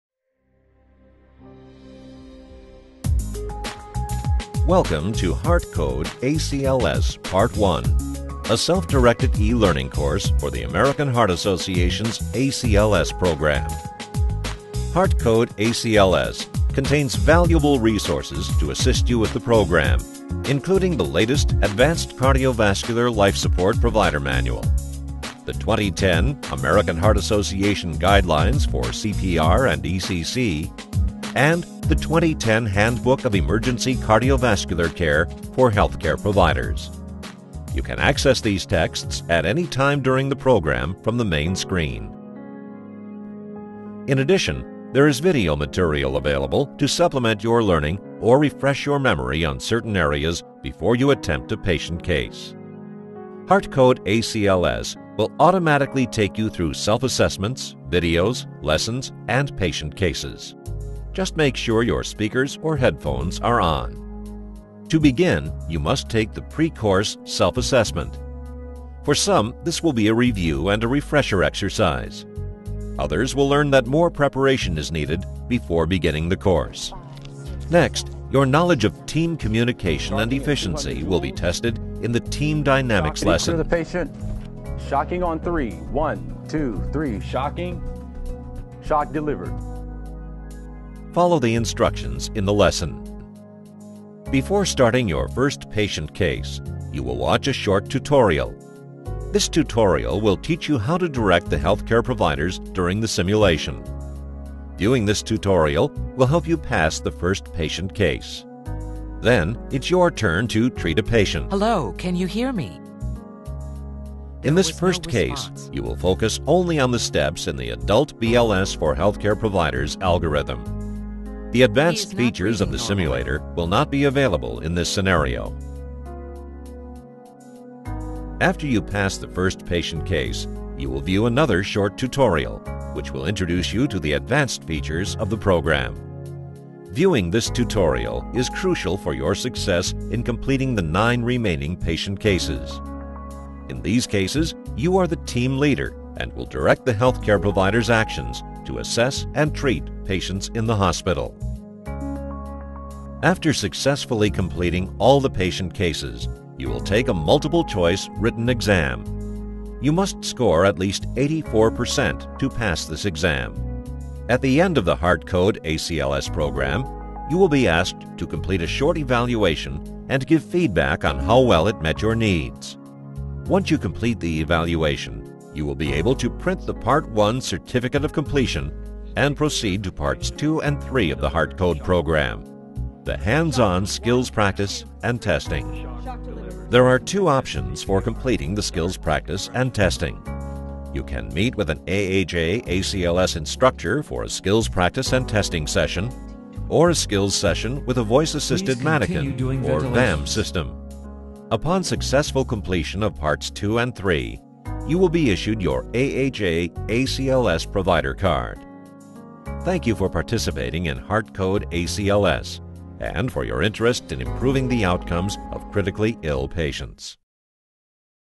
HeartCode® ACLS
HeartCode is a web-based, self-paced instructional program that uses eSimulation technology to allow students to assess and treat patients in virtual healthcare settings. Debriefings and coaching are provided immediately after each simulation to facilitate learning about basic and advanced life support.